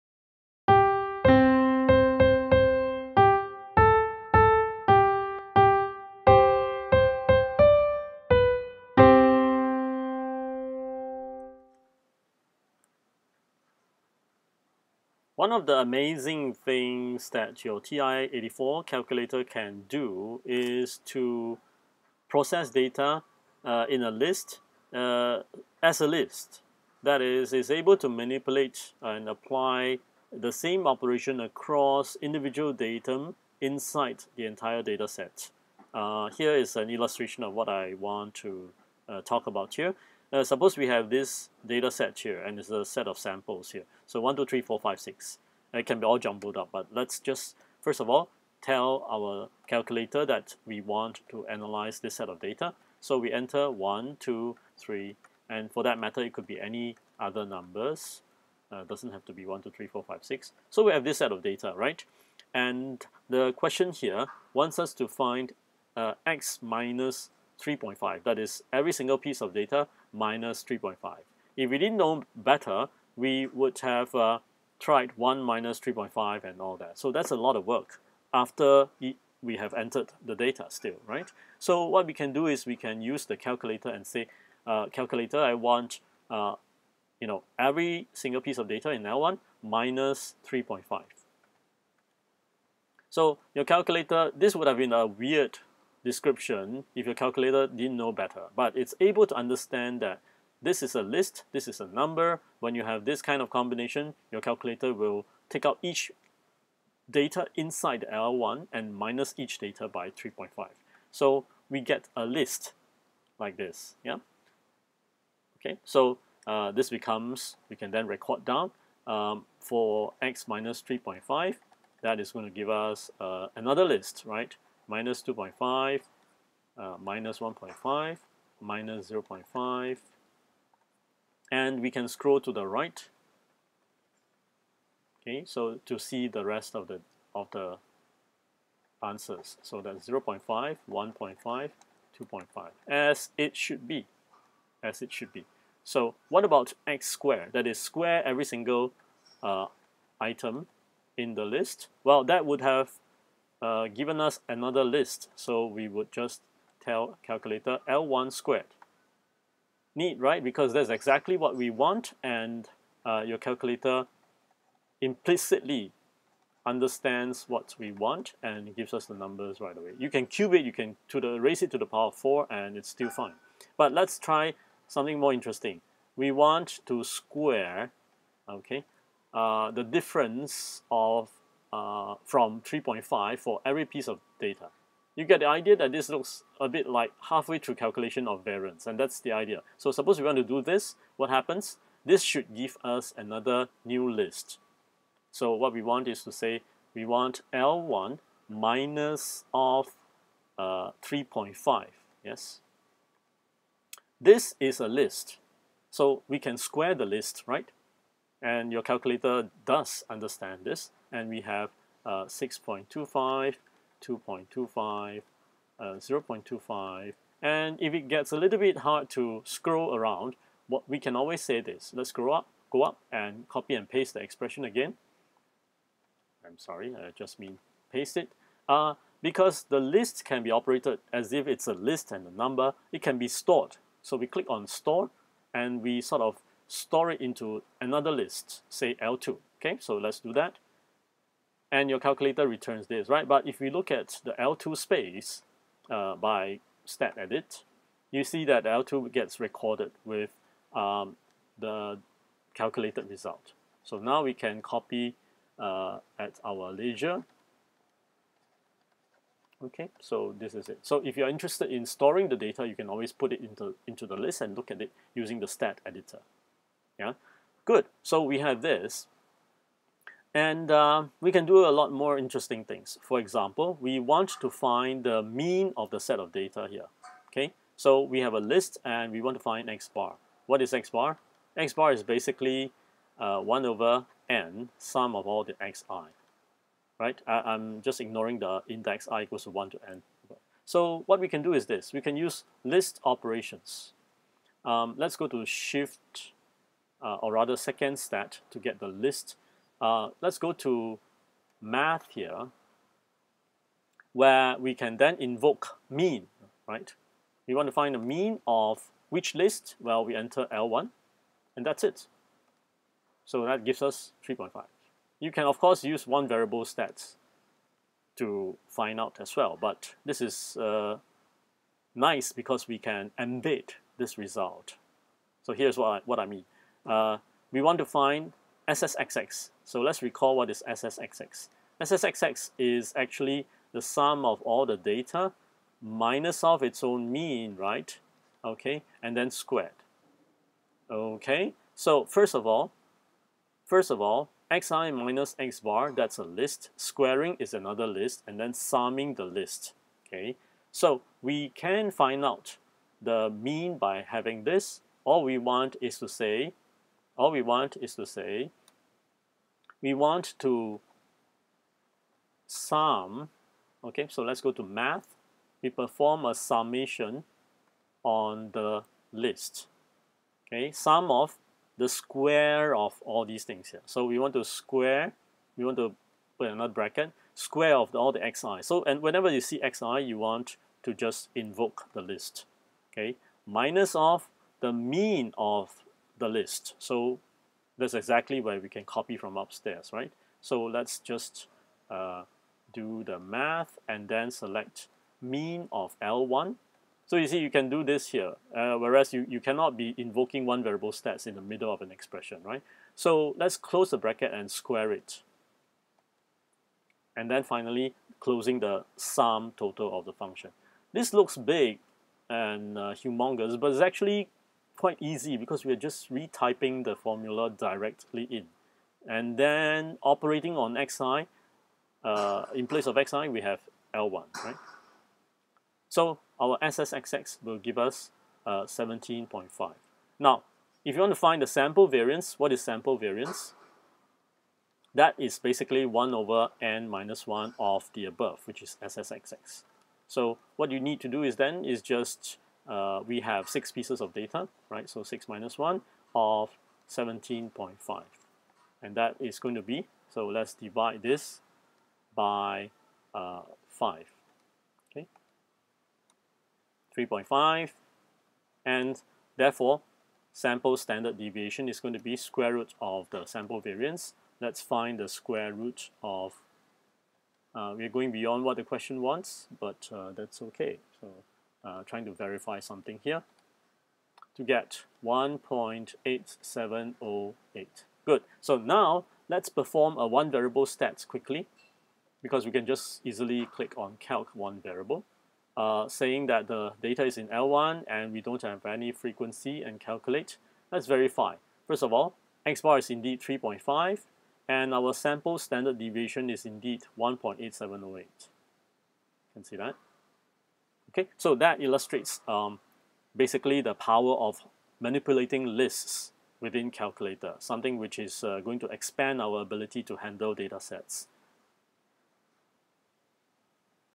Cool TI-84 List Operations
We need to manipulate data sets very frequently in statistical analysis, sometimes not using the standard calculations like mean and standard deviation.  In such cases, knowing how to manipulate the entire data using TI-84's native list operations makes you really efficient and stand out from the crowd.  In this video, we show how you can easily obtain sample variance directly using list operations as an illustration of how complicated formulas can be applied on data sets in TI-84.